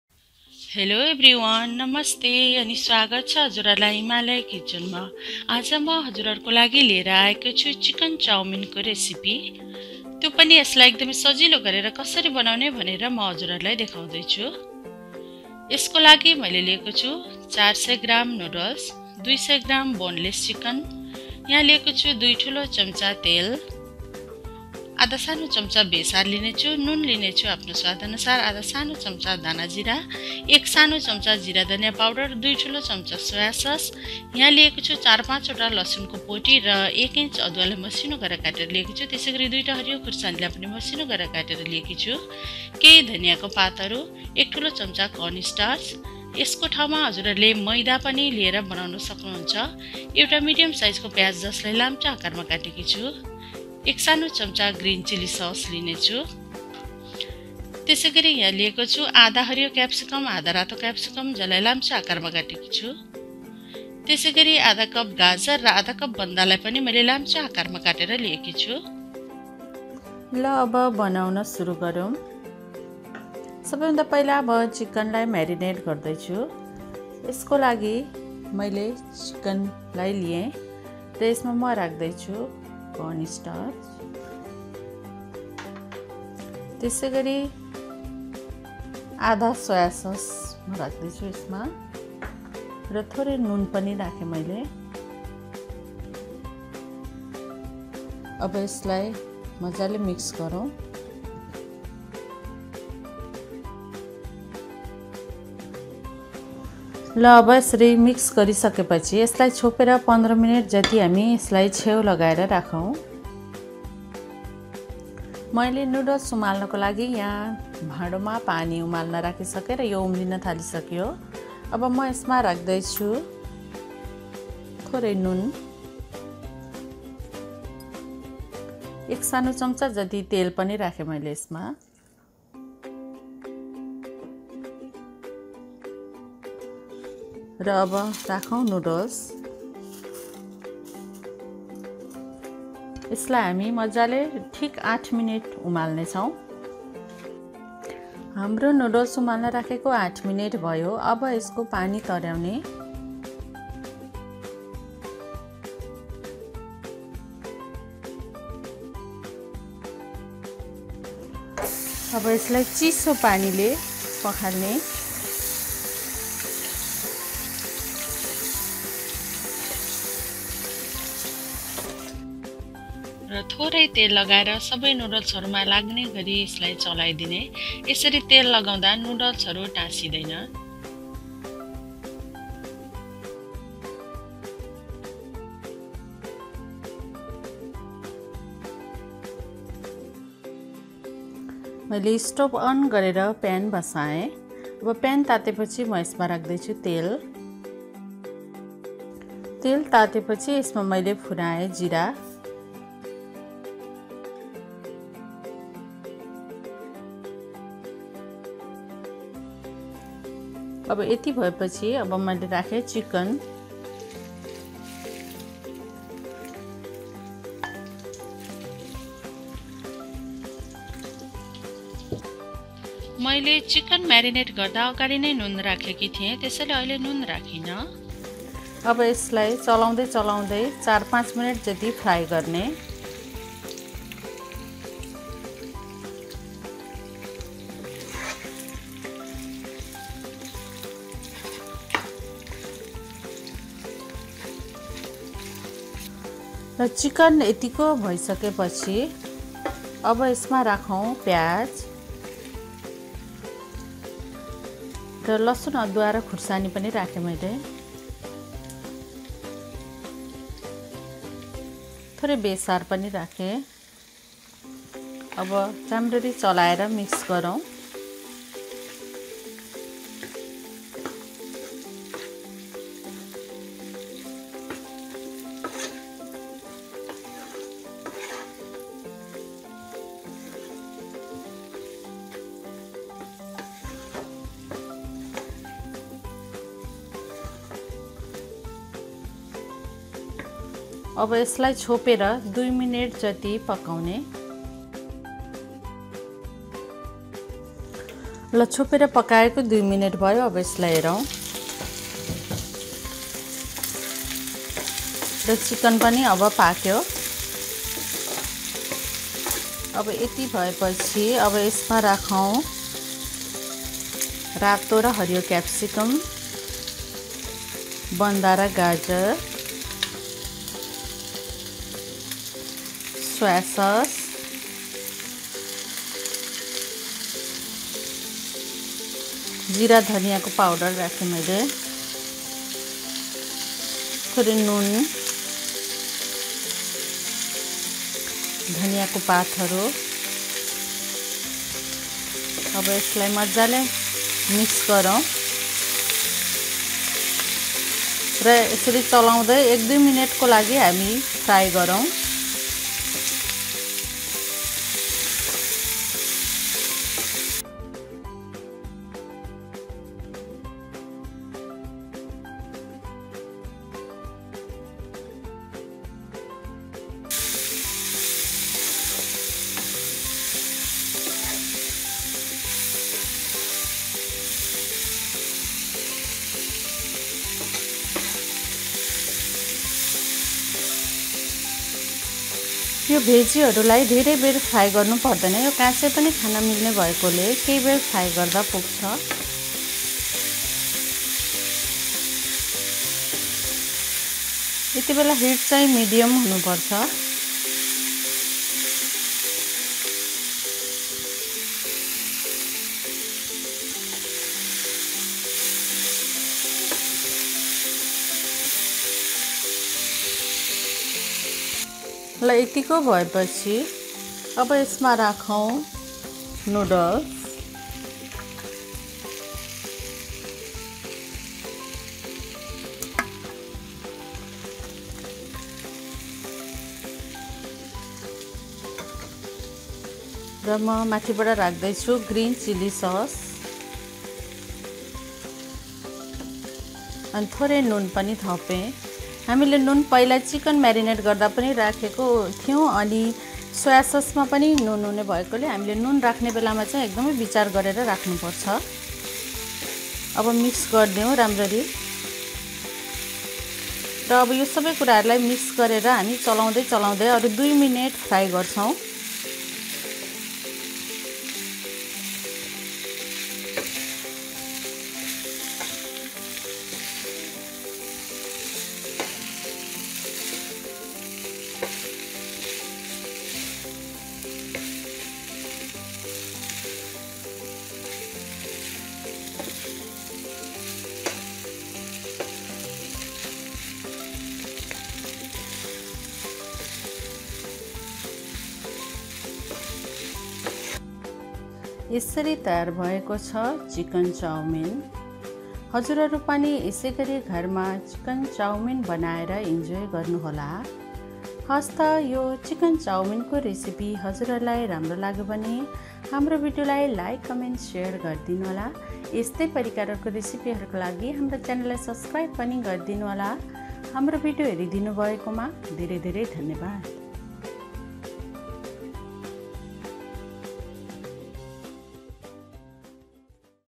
चिकन चाउमीन (Chicken Chow Mein) Recipe in Nepali [ENG Subtitles]
Learn how to make delicious chicken chow mein with this video!